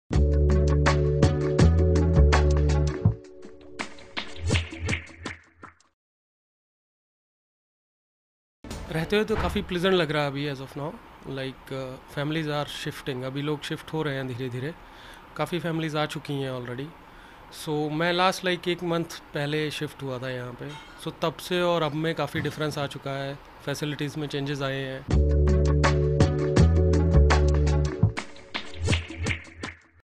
Happy Customer #22: Navdeep Kumar
Happy Customer #22: Navdeep Kumar shares his experience of residing in the modern integrated township of SARE Crescent ParC. He's happy with the way the project has developed. Watch the video for more info.